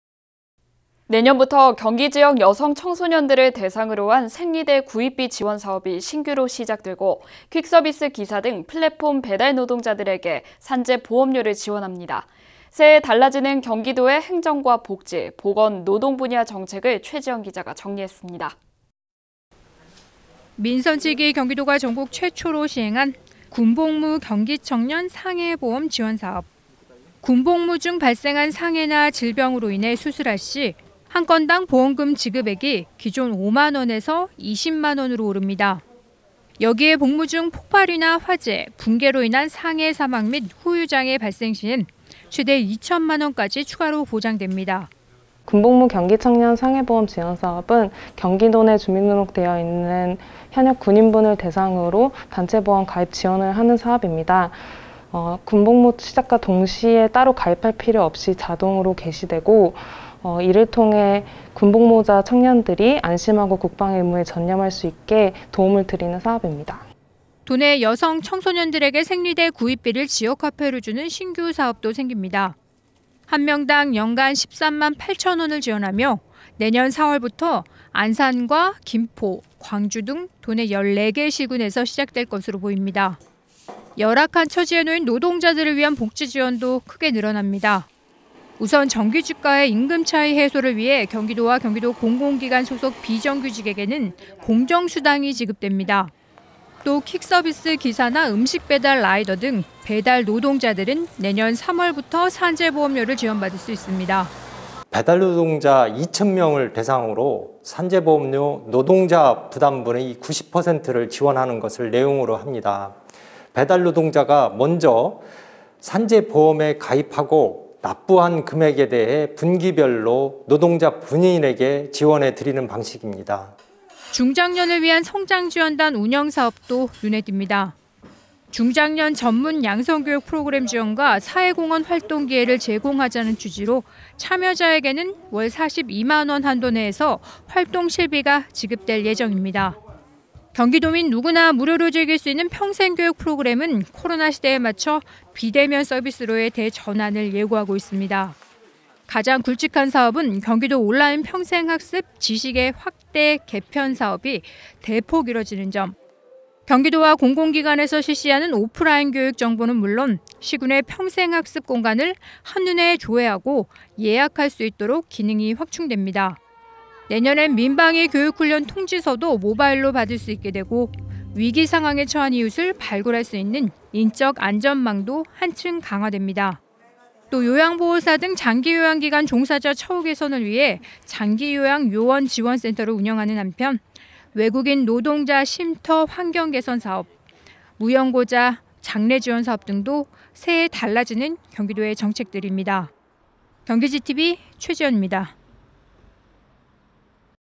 ① 새해 달라지는 경기도 생활은?
[앵커멘트]
내년부터 경기 지역 여성청소년들을 대상으로 한 생리대 구입비 지원 사업이 신규로 시작되고, 퀵서비스기사 등 플랫폼 배달노동자들에게 산재보험료를 지원합니다. 새해 달라지는 경기도의 행정과 복지, 보건, 노동 분야 정책을 최지현 기자가 정리했습니다.

[리포트]

민선 7기 경기도가 전국 최초로 시행 한 군복부 경기청년 상해보험 지원 사업.
군복무 중 발생한 상해나 질병으로 인해 수술을 할 시, 1건 당 보험급 지급액이 기존 5만원에서 20만 원으로 오릅니다.
여기에 복무 중 폭발이나 화재, 붕괴로 인한 상해사망 및 후유장해 발생 시엔 최대 2천만 원까지 추가로 보장됩니다.
[인터뷰]이금비/경기도 청년복지정책과 주무관
군복무 경기청년 상해보험 지원 사업은 경기도 내 주민등록돼 있는 현역 군인들을 대상으로 전체 보험 가입 지원을 하는 사업입니다. 군복무 시작과 동시에 따로 가입할 필요 없이 자동으로 개시되고, 이를 통해 군복무 청년들이 안심하고 국방의 의무에 전념할 수 있게 도움을 드리는 겁니다.

도내 여성청소년들에게 생리대 구입비를 지역화폐로 주는 신규 사업도 생깁니다.
한 명당 연간 13만8천 원을 지원하며, 내년 4월부터 안산과 김포, 광주 등 도내 14개 시군에서 시작될 것으로 보입니다.

열악한 처지에 놓인 노동자들을 위한 복지 지원도 크게 늘어납니다.
우선 정규직과의 임금차이 해소를 위해 경기도와 경기도 공공기관 소속 비정규직에게는 ‘공정수당’이 지급됩니다.

또 퀵서비스 기사나 음식 배달 라이더 등 배달노동자들은 내년 3월부터 산재보험료를 지원받을 수 있습니다.
[인터뷰]강현석/경기도 노동권익과장
배달노동자 2천 명을 대상으로 산재보험료 노동자 부담금의 90%를 지원하는 것을 내용으로 합니다. 배달노동자가 먼저 산재보험에 가입하고 납부한 금액에 대해 분기별로 노동자 본인에게 지원해드리는 방식입니다.

중장년을 위한 성장지원단 운영 사업도 눈에 띕니다.
중장년 전문 양성교육 프로그램 지원과 사회공헌 활동기회를 제공하자는 취지로, 참여자에게는 월 42만 원 한도 내에서 활동실비가 지급 될 예정입니다.

경기도민 누구나 무료로 즐길 수 있는 ‘평생교육프로그램’은 코로나 시대에 맞춰 비대면 서비스로의 대전환을 예고하고 있습니다.
가장 굵직한 사업은 경기도온라인평생학습 ‘지식’ 의 확대, 개편 사업이 대폭 이뤄지는 점.
경기도와 공공기관에서 실시하는 오프라인교육 정보는 물론 시군의 평생학습공간을 한눈에 조회하고 예약할 수 있도록 기능이 확충됩니다.

내년엔 민방위 교육훈련통지서도 모바일로 받을 수 있게 되고, 위기상황에 처한 이웃을 발굴할 수 있는 인적안전망도 한층 강화됩니다.
또 요양보호사 등 장기요양기관 종사자 처우개선을 위해 ‘장기요양요원지원센터’를 운영하는 한편 외국인 노동자 쉼터 환경 개선 사업, 무연고자 장례 지원 사업 등도 새해 달라지는 경기도의 정책들입니다. 경기GTV 최지현입니다.
영상취재: 서경원, 영상편집: 강윤식

[자막]
박스자막)1.군복무 경기청년 상해보험 혜택 확대
2. [인터뷰]이금비/경기도 청년복지정책과 주무관
박스자막)3.여성청소년 보건위생물품 지원
박스자막)4. 비정규직 공정수당 지급
박스자막)5.배달노동자 산재보험료 지원
6.[인터뷰] 강현석/경기도 노동권익과장
박스자막)7.중장년 생애주기별 성장지원단 운영
박스자막)8. 평생학습포털 지식 확대‧개편